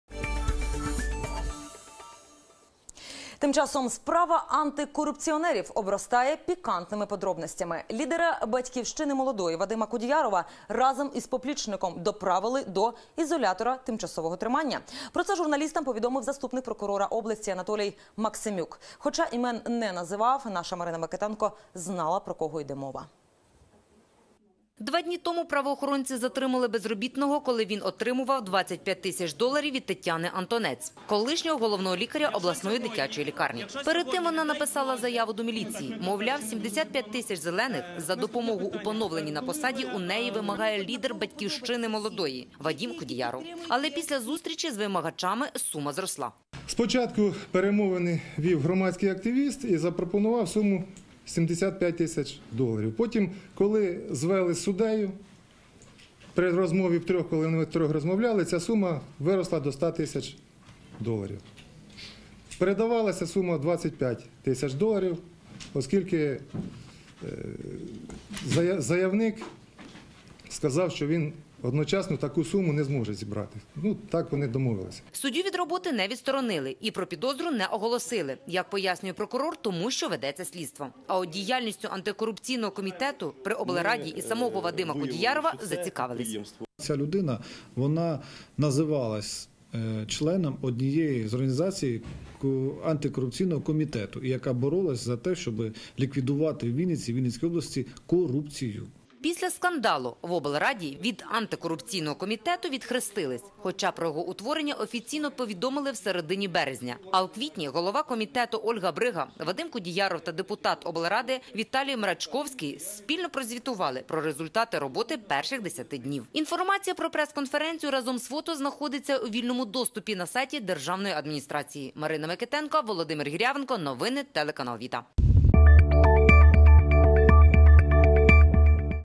Телеканал ВІТА новини 2014-06-27 Антикорупціонера затримали за хабар
Справа антикорупціонерів обростає пікантними подробицями. Лідера «Батьківщини молодої» Вадима Кудіярова разом із поплічником доправили до ізолятора тимчасового тримання. Про це журналістам повідомив заступник прокурора області Анатолій Максимюк.